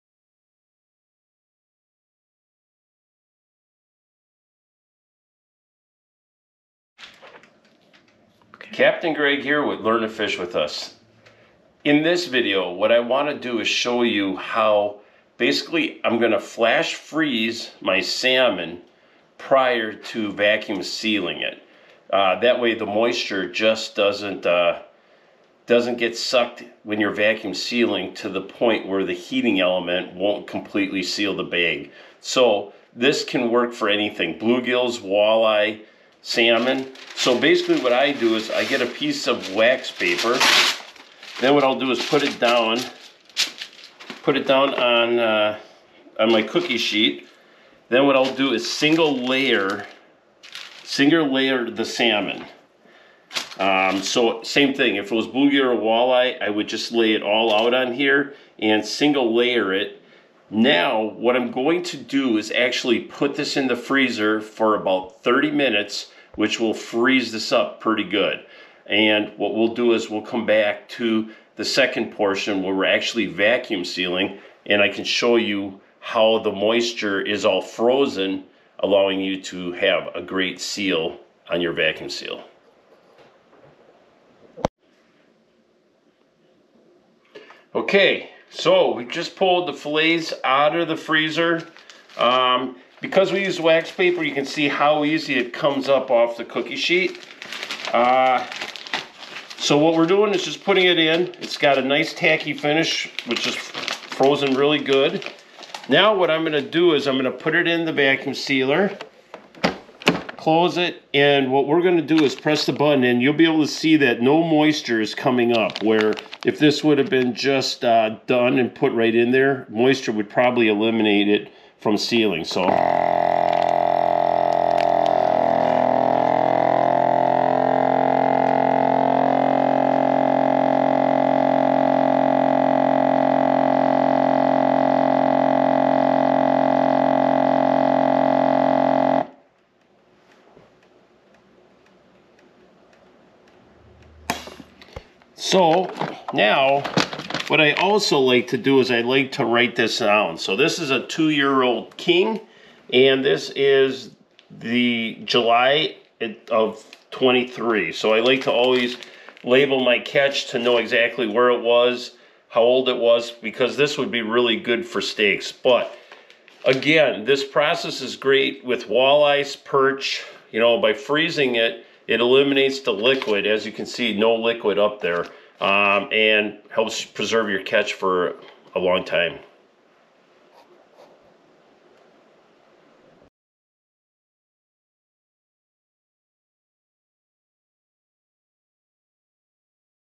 How To Easily Vacuum Seal Fish Along With Tips
This video shows you 'How To' easily vacuum seal fish without the bag not sealing